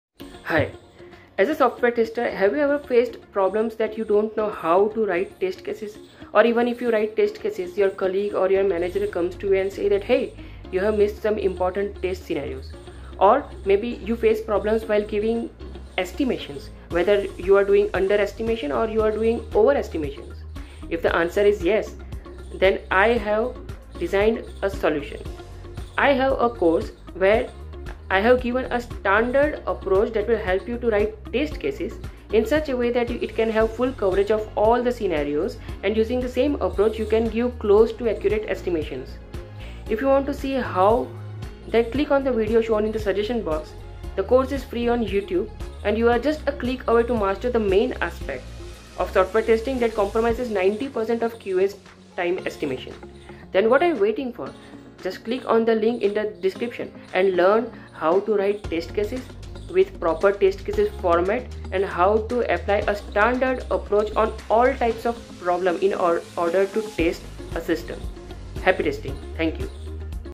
Master the Art of Software Testing: Join Our Comprehensive Course Today!
As a software tester, have you faced problems that :
1) You don't know how to write test cases
2) Or even if you write test cases, your manager or colleague comes to you and complains that you have missed some important scenarios
3) Or maybe you face problems while giving estimations?//either you are doing underestimation or overestimation about your tasks

If the answer is YES, then, I have a solution. I have designed a course where I have given a standard approach that will help you to write test cases in such a way that it covers all the test scenarios, and using the same approach, you can give close to accurate estimations.

If you want to see how, then click on the video shown in the suggestion video box.  The course is free on YouTube, and you are just a click away from mastering the main aspect of software testing that compromises 90% of QA's time.

Happy Testing..